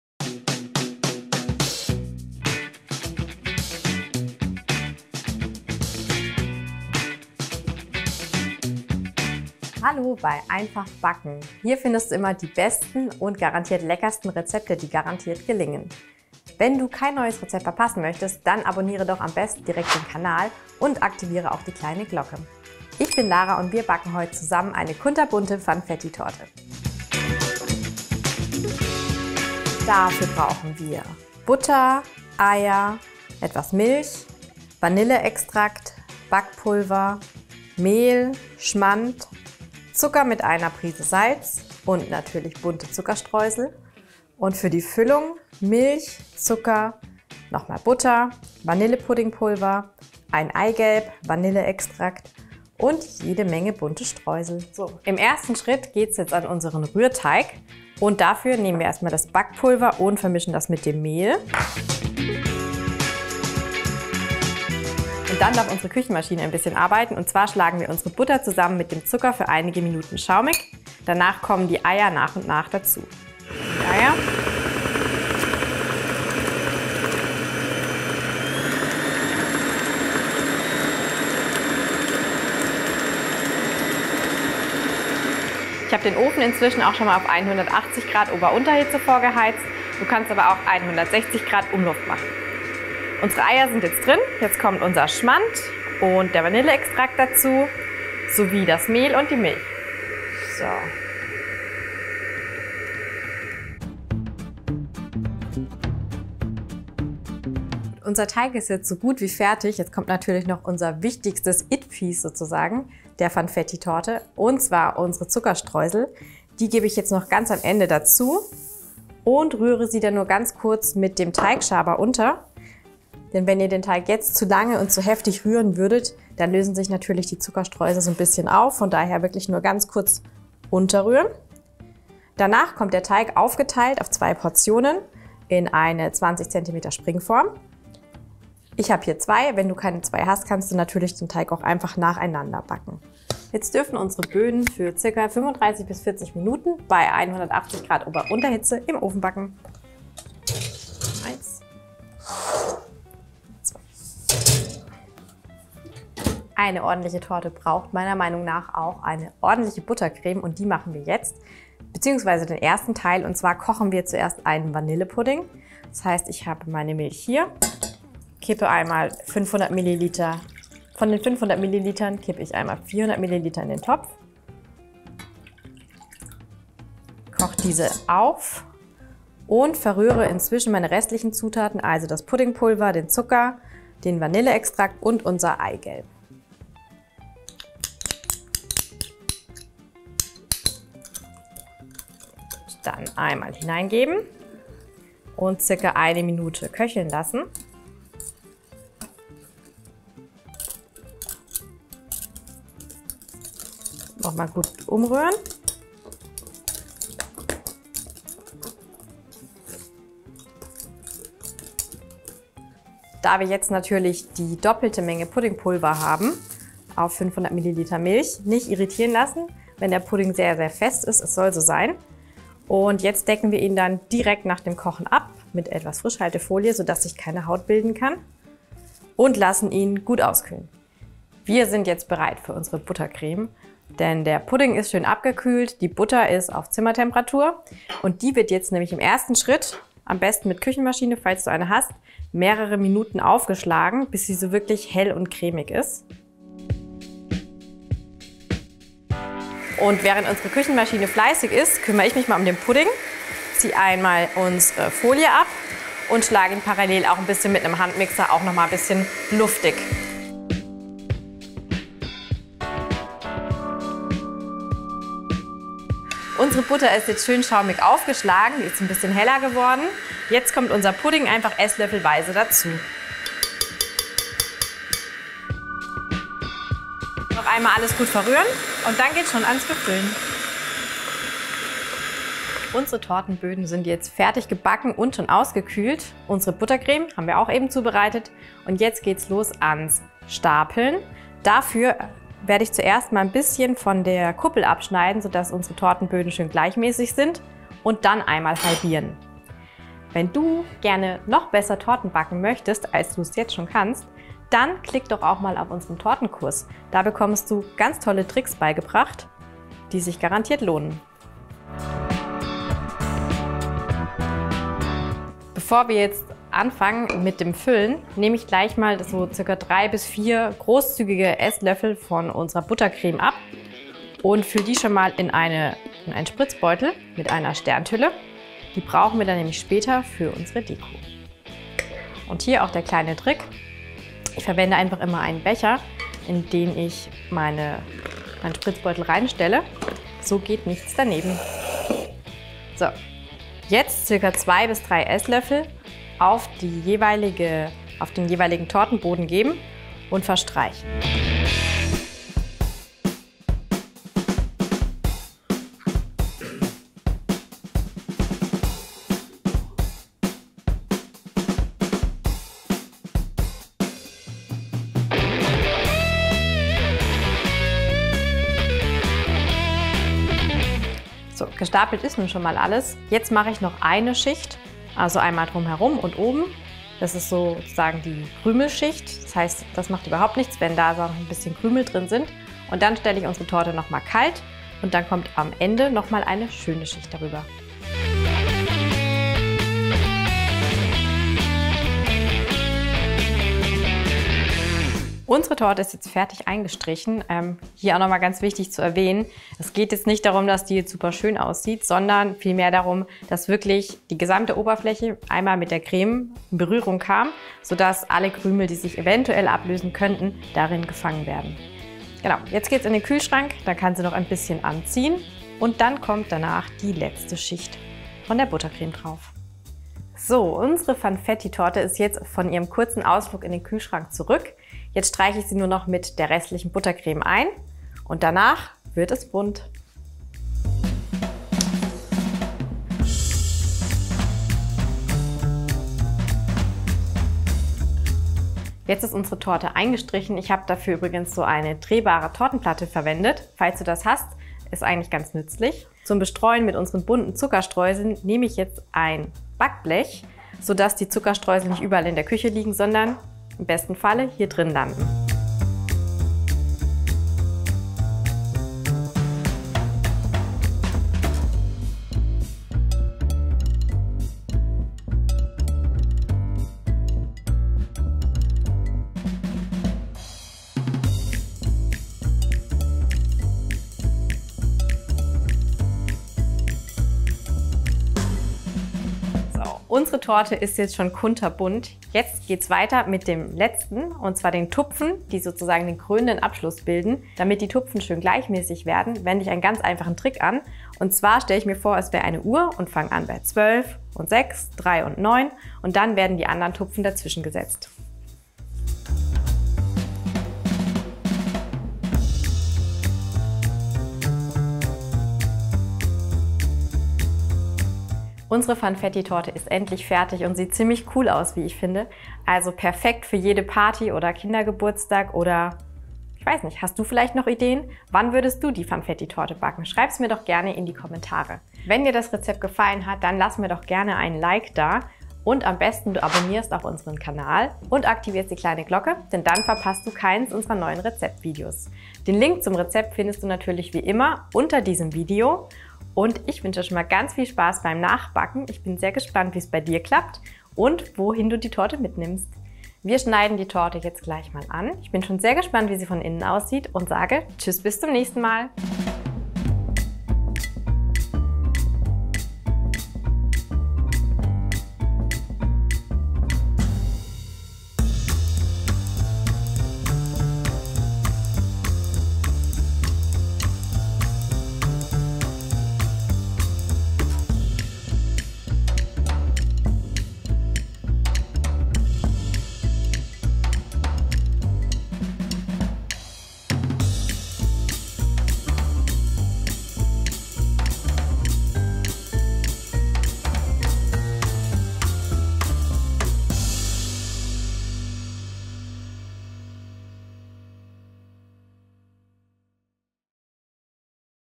Funfetti-Torte | Konfetti Torte 🥳 | Bunte Geburtstagstorte | Einfach Backen
🎉🎊Die perfekte Geburtstagstorte für Kinder oder Erwachsene ist unsere bunte Funfetti-Torte! 🎂 Mit diesem Rezept aus einem einfachen Rührteig und einer köstlichen Buttercreme wird dir die Konfetti-Torte mit bunten Streuseln auf jeden Fall gelingen.🎁🎈🎉

🗒️ Unsere Zutaten
für die Rührteigböden:
400 g Weizenmehl (Type 405)
1 Pck. Backpulver
1 Prise Salz
250 g weiche Butter (zimmerwarm)
180 g Zucker
3 Eier (Gr. M)
200 g Schmand
2 TL Vanilleextrakt
5 EL Milch
100 g bunte Zuckerstreusel
Etwas Butter für die Formen

Für Buttercreme & Dekoration
500 ml Milch
1 TL Vanilleextrakt
2 Pck. Vanillepuddingpulver (à 37 g Pulver)
100 g Zucker
1 Eigelb (Gr. M)
500 g weiche Butter (zimmerwarm)
200 g bunte Zuckerstreusel

*🎂 Unser Geheimtipp – Drehbare Tortenplatte
*🍰 Praktisches Palettenmesser-Set

Hat dir das Video gefallen? Du willst kein neues Rezept mehr verpassen?